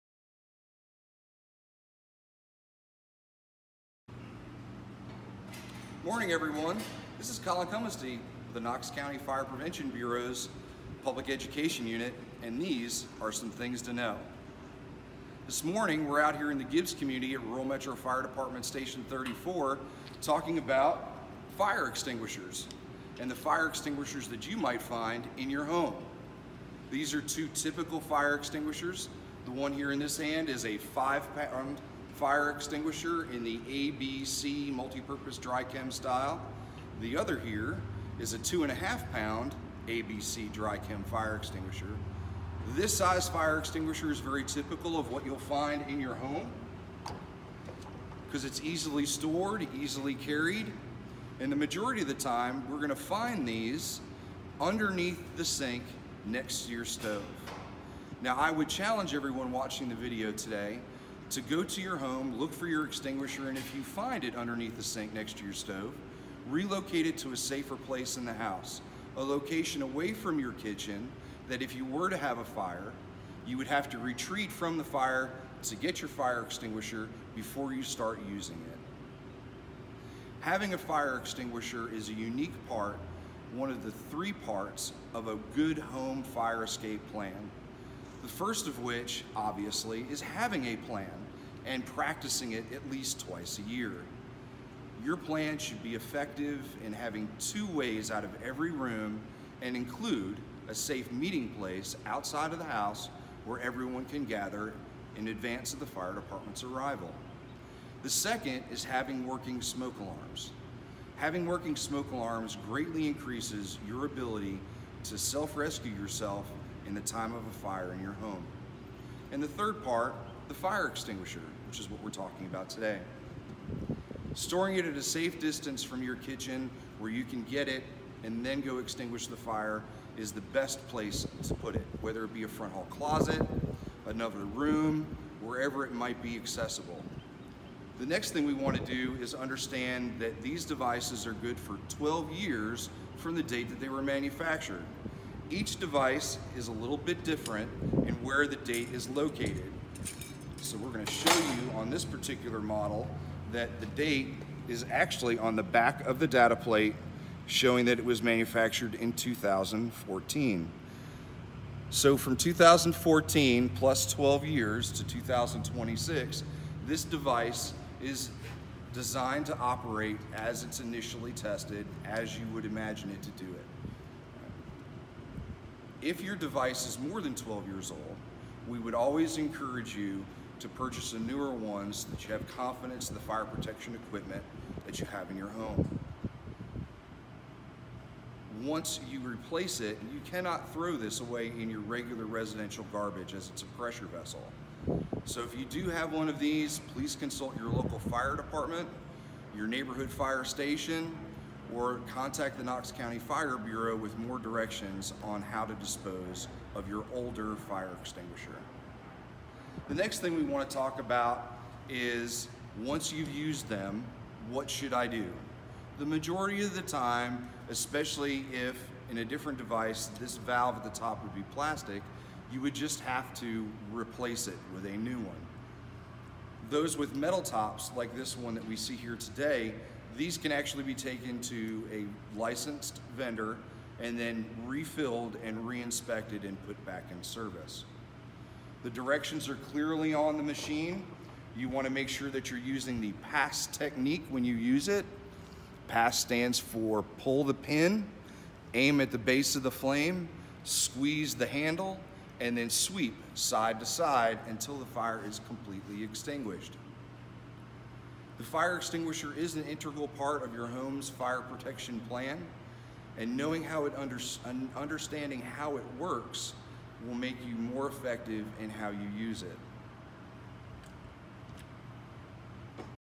Things To Know, Home Fire Extinguishers
In this segment of "Things to Know...", The Bureau's Colin Cumesty talks to us about home fire extinguishers and where you should be keeping them in your home.